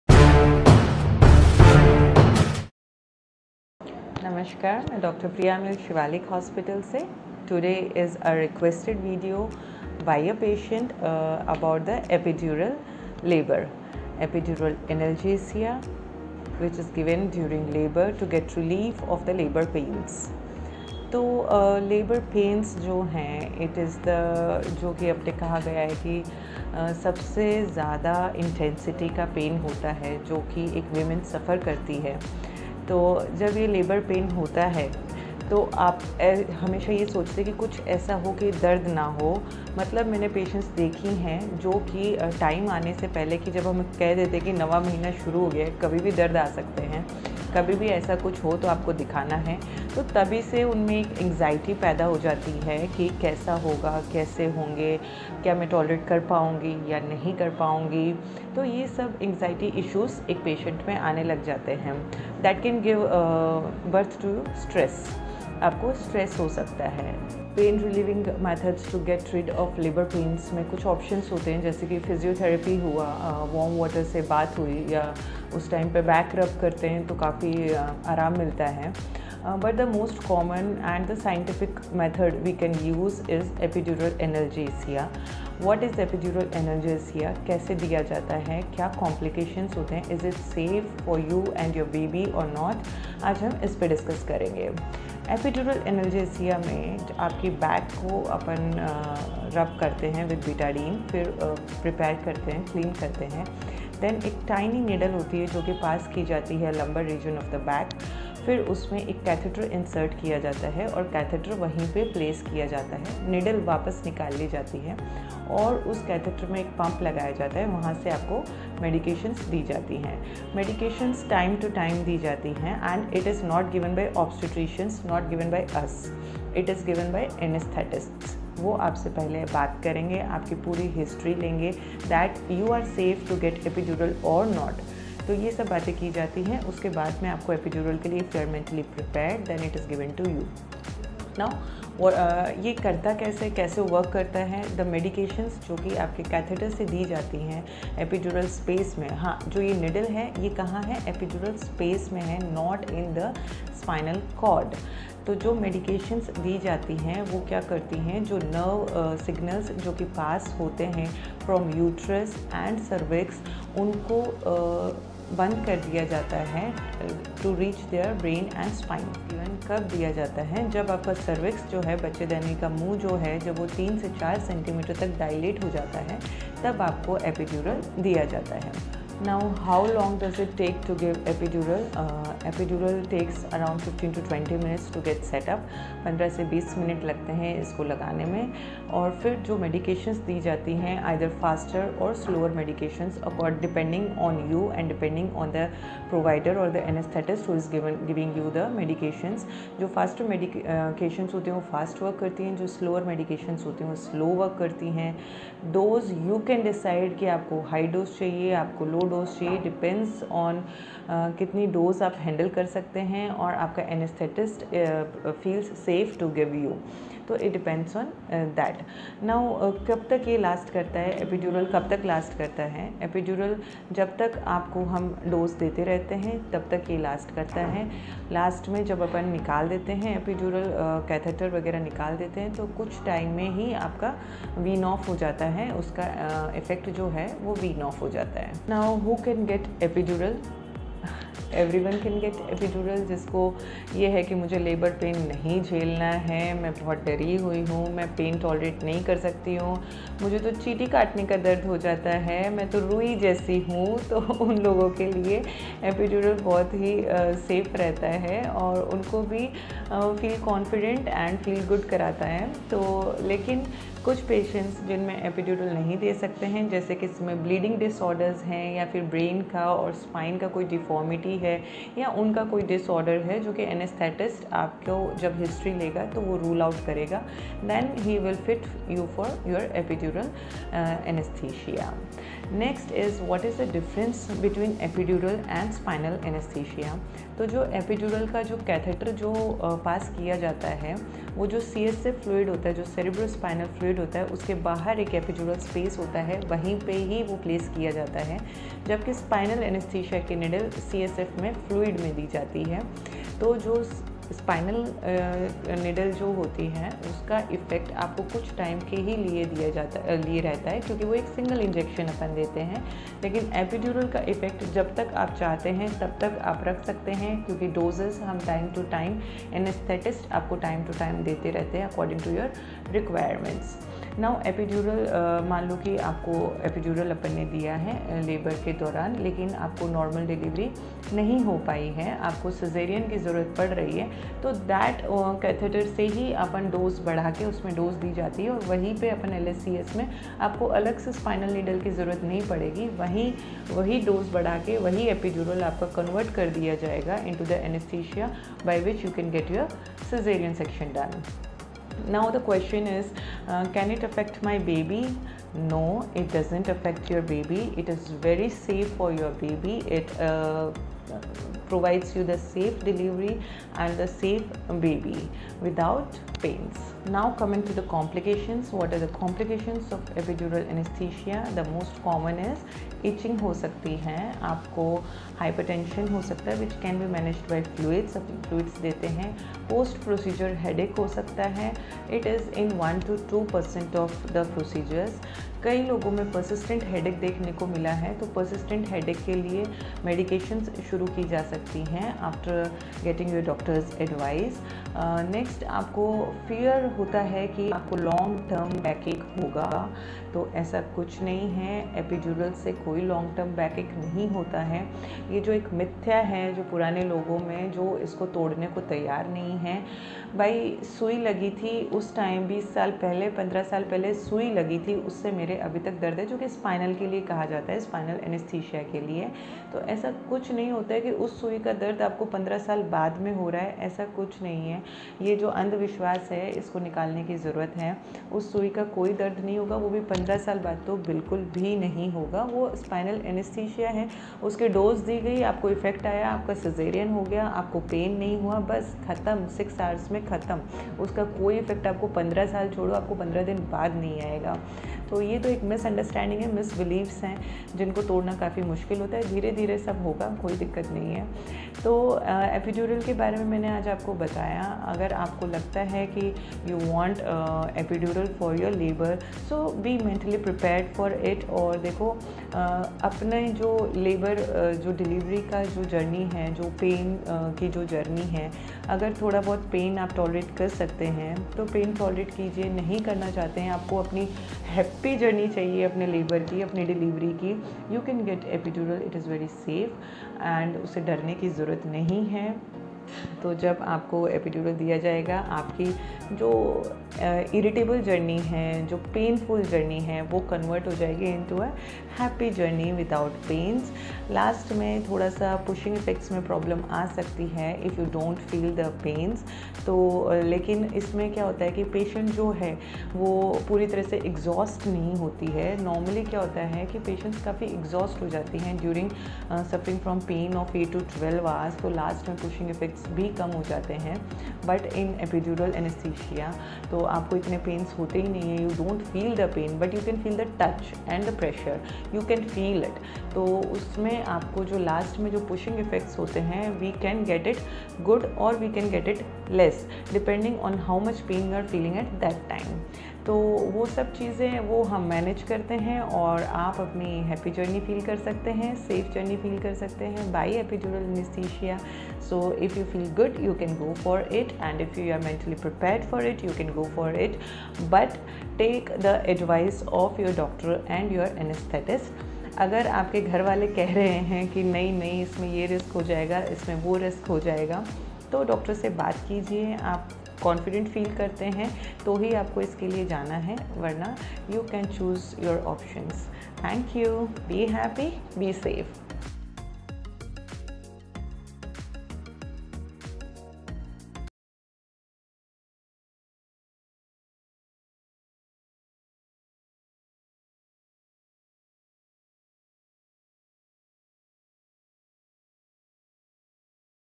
What is Painless Delivery? I बिना दर्द के Delivery | Dr. Priya Meel Ranwa
Painless delivery also called as labor  analgesia is  a procedure  wherein epidural analgesia  is  given so that the patient  is kept pain free  throughout the normal delivery. In this the procedure is similar to that of  spinal anesthesia except that in this the catheter  is inserted through the  epidural needle  and the needle is removed and the catheter remains in the back till the delivery   is  conducted.
Watch this video, as Dr Priya Meel Ranwa, Consultant-  Gynecologiest at the Shivalik Hospital Sikar Best Gynec Hospital in Shekhawati, explains Painless delivery Methods, Complications & Advantages.
00:03:11 Introduction
02:50:25 How It Works
03:10:00 When It Given